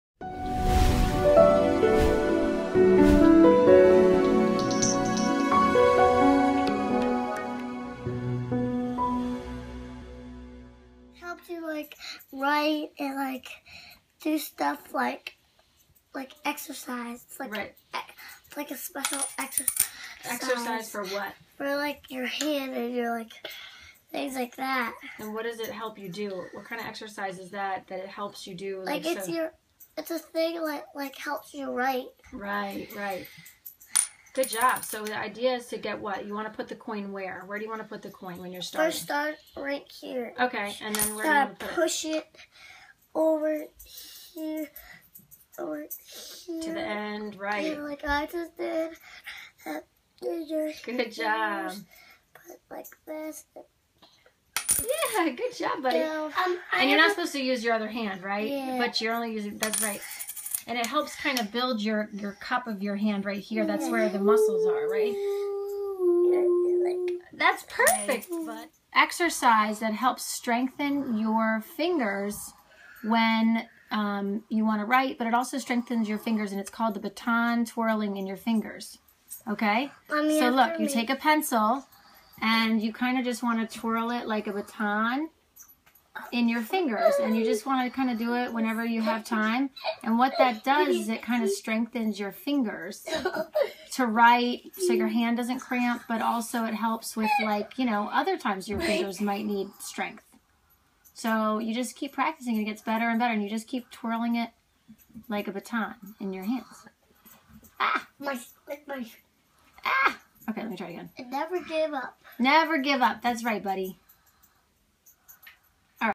PT/OT exercises to build hand & finger strength
PT/OT exercises to build hand & finger strength for kids and adults to help with writing & other activities that we use our hands & fingers for.